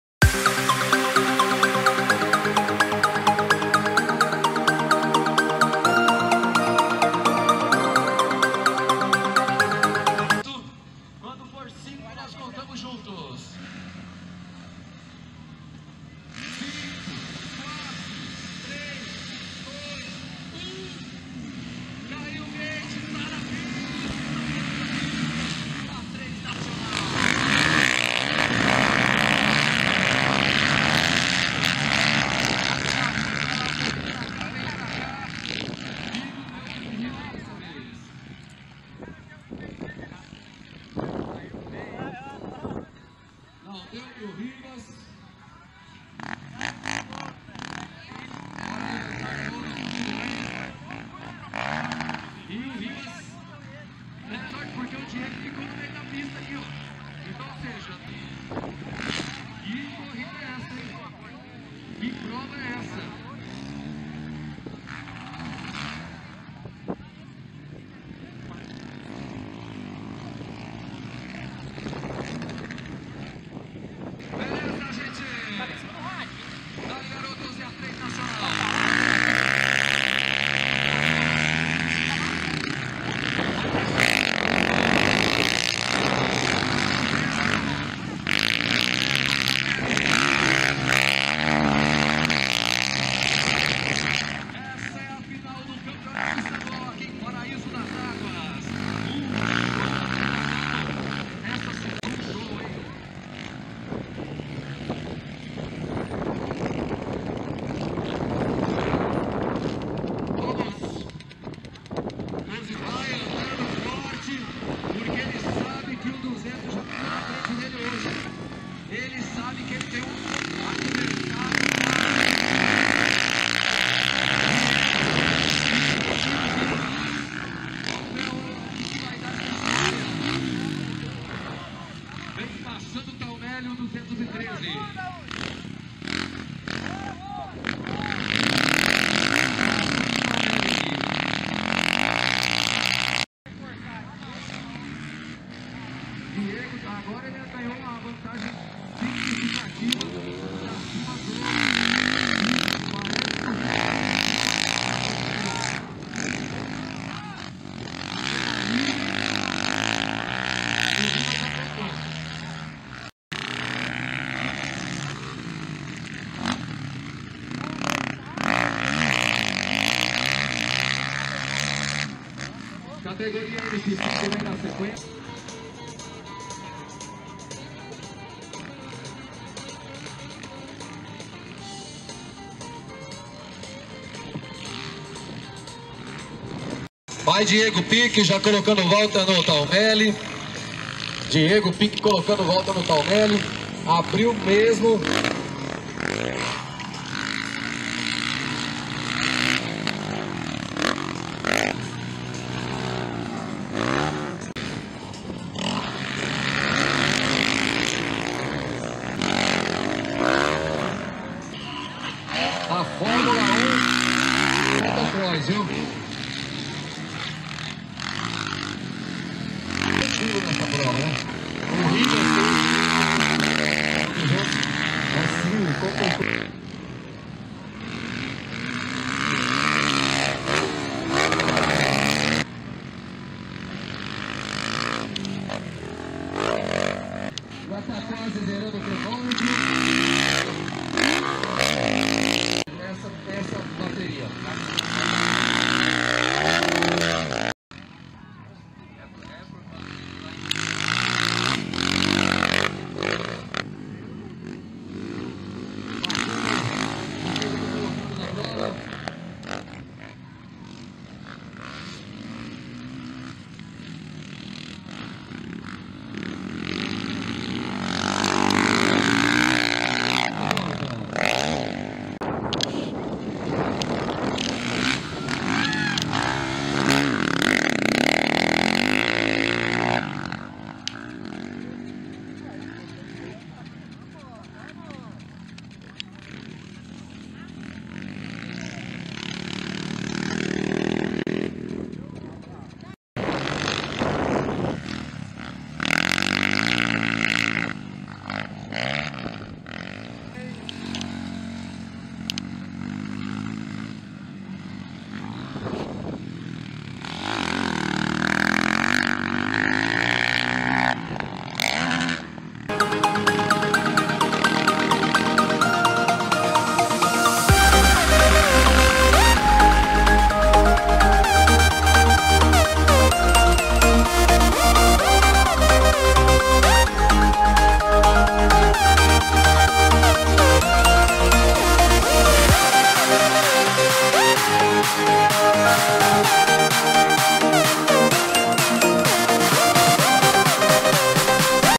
MX3 Nacional em Paraíso das Águas - Estadual Sul-mato-grossense de Motocross 2023
Bateria disputada no domingo (29) em Paraíso das Águas (MS).

Acompanhe o velocross e motocross Sul-mato-grossense no blog @TVHoleshot

Confira o gate: EM BREVE